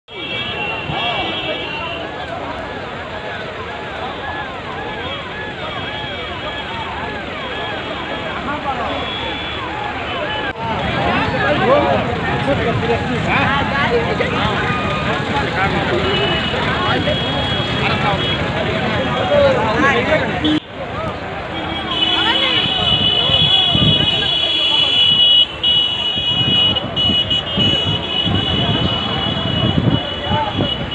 Ara- Buxar मुख्य मार्ग पर पलटी BUS ,मच गया हाहाकार, गूंजने लगी चीख-पुकार
आरा-बक्सर मुख्य मार्ग पर एक दर्दनाक हादसा हुआ है। उदवंतनगर थाना क्षेत्र के बड़कागांव के पकड़ीयबर पुल के पास बस पलट गयी। इस हादसे में एक की मौत हो गयी है, वहीं दर्जनों लोग गंभीर रूप से जख्मी हो गये हैं।

बताया जा रहा है कि बस करनामेपुर से आरा जा रही थी, तभी ये दर्दनाक हादसा हुआ। इस हादसे में एक बाइक सवार को भी गंभीर चोटें आयी हैं। फिलहाल स्थानीय लोगों की मदद से जख्मी मुसाफिरों को राहत पहुंचायी जा रही है।